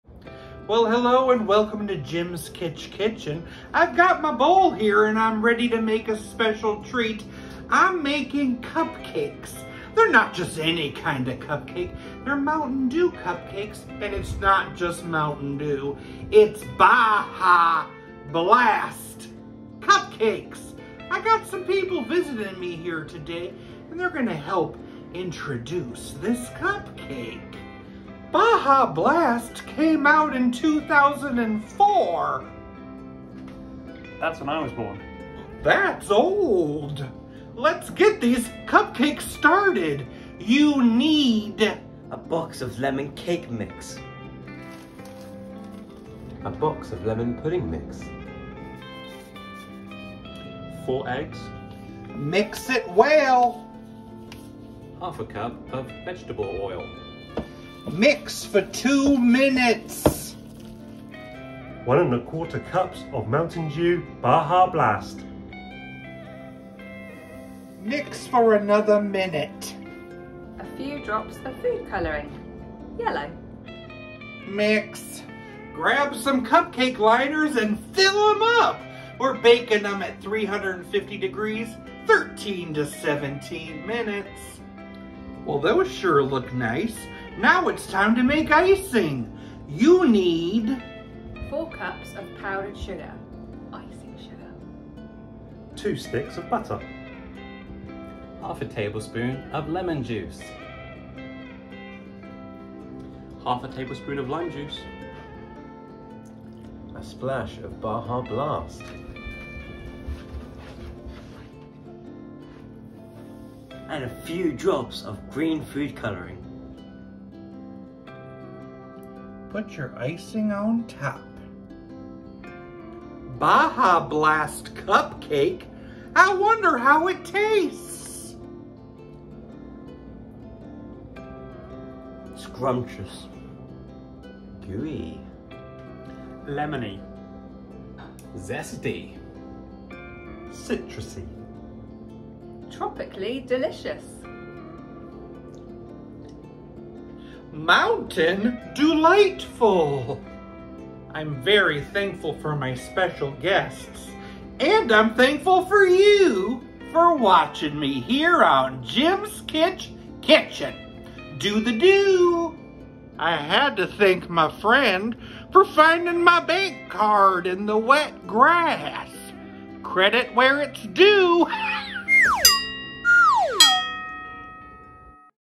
How to make Baja Blast Mountain Dew Cupcakes
Baja Blast Mountain Dew Cupcakes

Time for a really wild recipe!

You need:

1 box lemon cake mix 1 box lemon pudding mix4 Eggs1 1⁄4 cups Mountain Dew Baja blast  soda 1⁄2 cup vegetable oil 3 -5 drops yellow food coloring 4 cups powdered sugar2 sticks of butter1⁄8 cup Mountain Dew Baja blast soda 1⁄2 tablespoon lemon juice1⁄2 tablespoon lime juice3 -5 drops green food coloring

courtesy of the YouTube music library

Here is a link to my channel if you want to see more!!!

If you are interested in collaborating on a video, or for me to review a product or just to say hi!